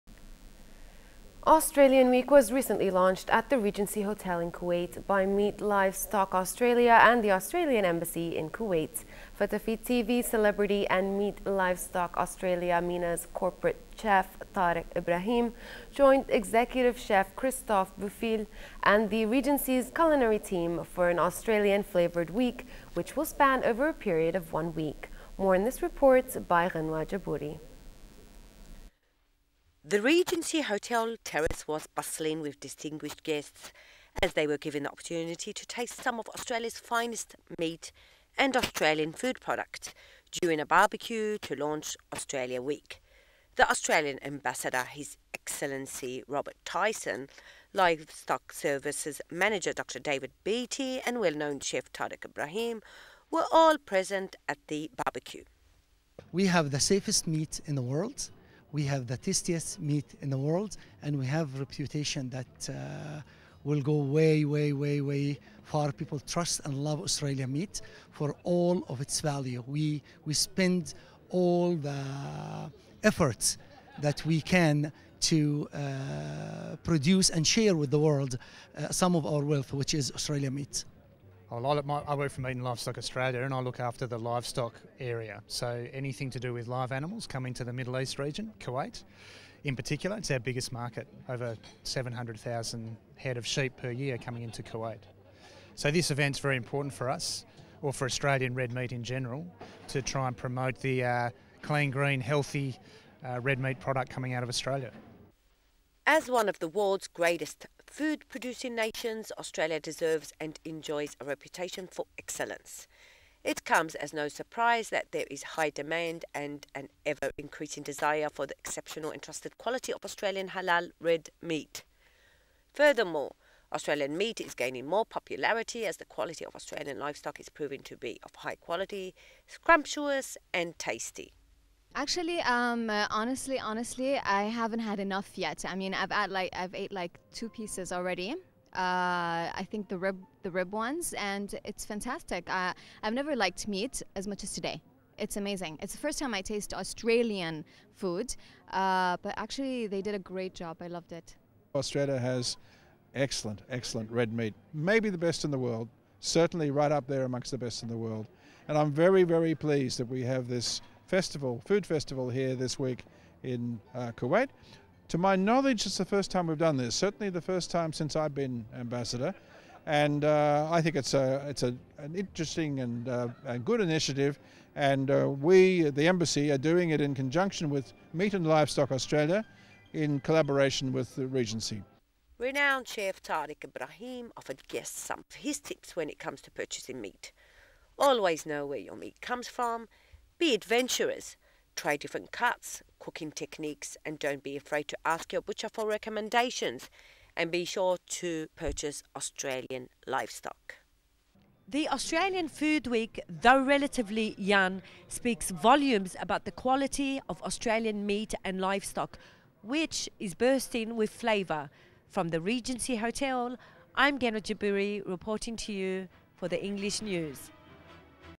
Australian Embassy in Kuwait hosts Australia Week featuring celebrity & world-renowned chefs
Australian week was launched at the Regency Hotel, by "Meat Live Stock Australia," the Australian Embassy and in collaboration with the Regency Hotel. Fatafeet TV celebrity and Meat Livestock Australia MENA's corporate chef, Tarek Ibrahim joined executive Chef Christophe Buffille and the Regency's culinary team for an Australian flavored week.